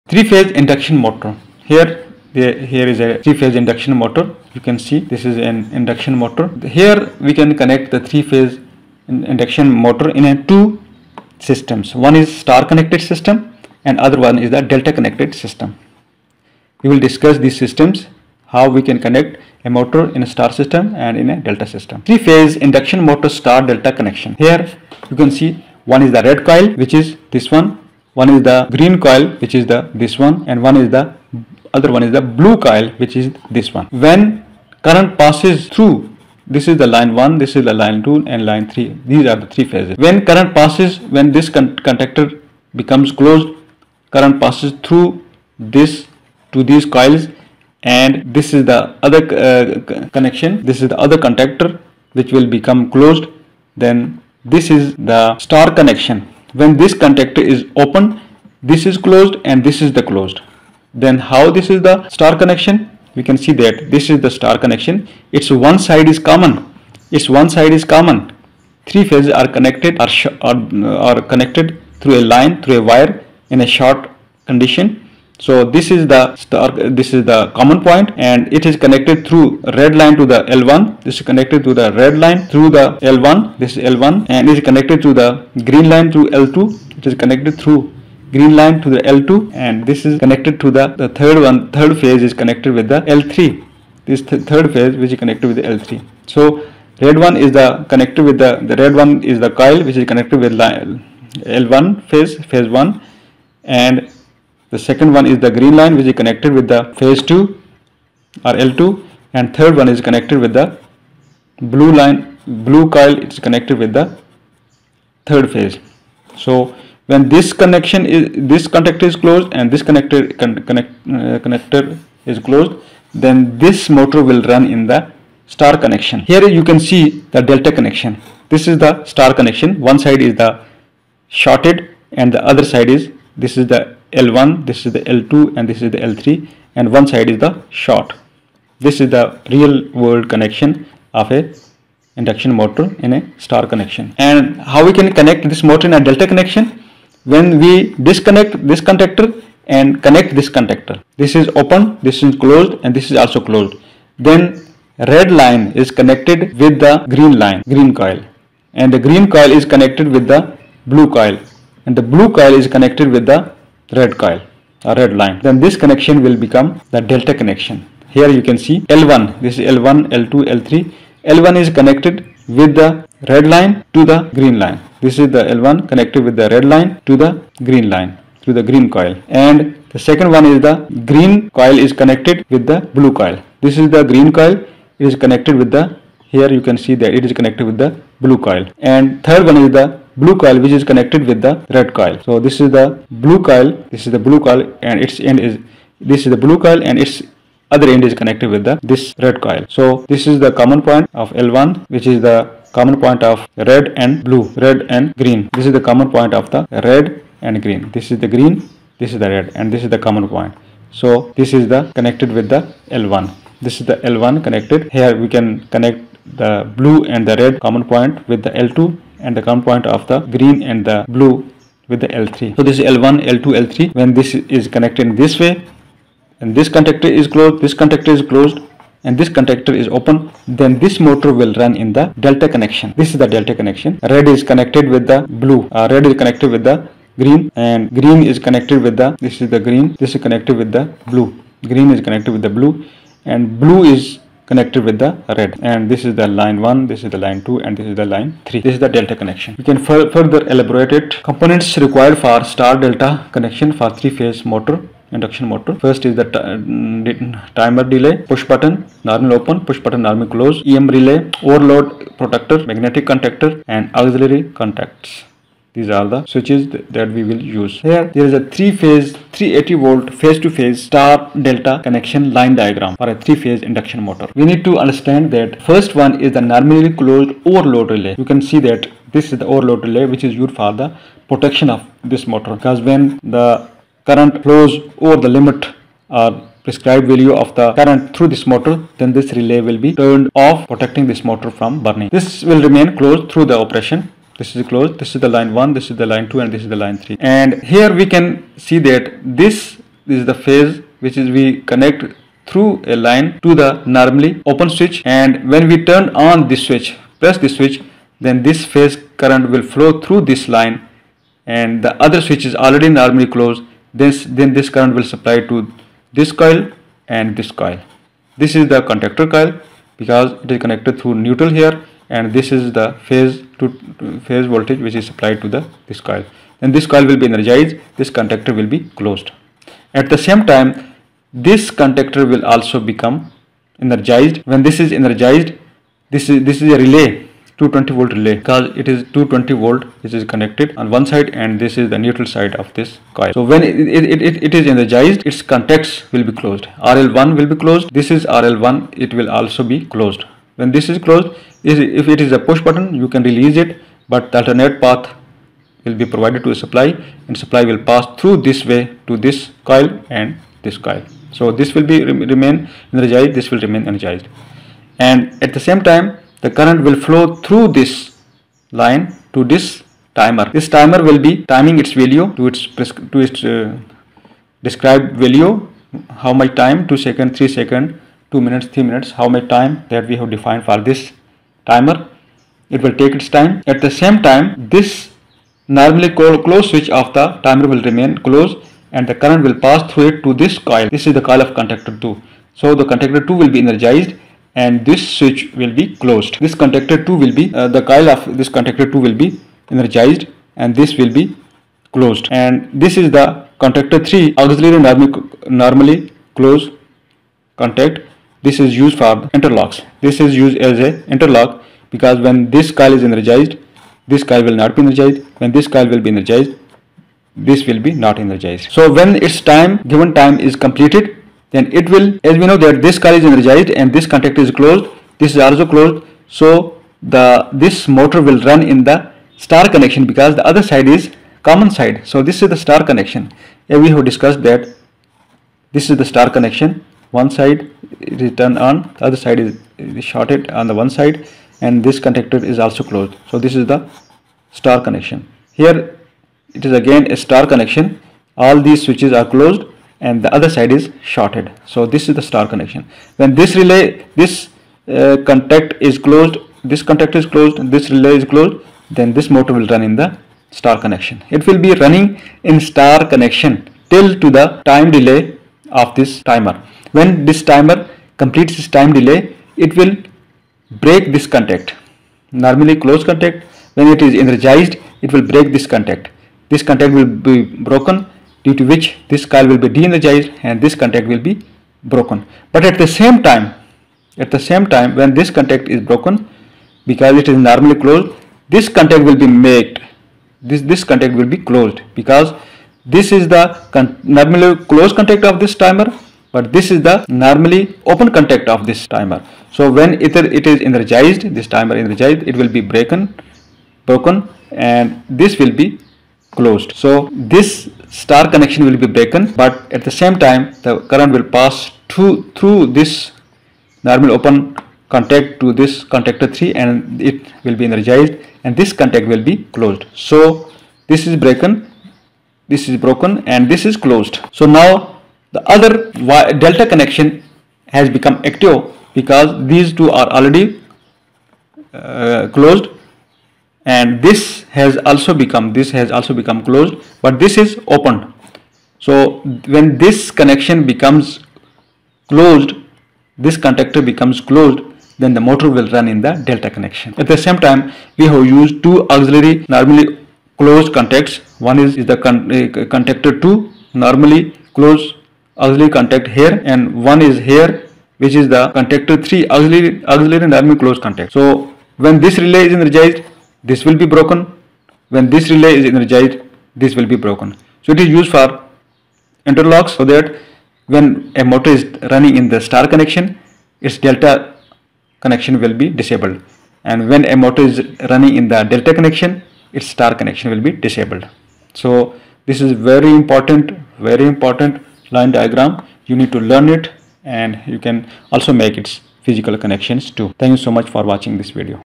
Three phase induction motor star delta connection explained @Tech Reveals
In this comprehensive video, we discuss the star delta connection method for three phase induction motors. Explore the principles behind this technique, and implementation considerations. Whether you're a student of electrical engineering or a seasoned professional in the field, this resource aims to clarify the concept and its practical implications in motor control and power systems. Dive deep into the world of three phase motors and discover how star delta connection plays a crucial role in optimizing performance and efficiency.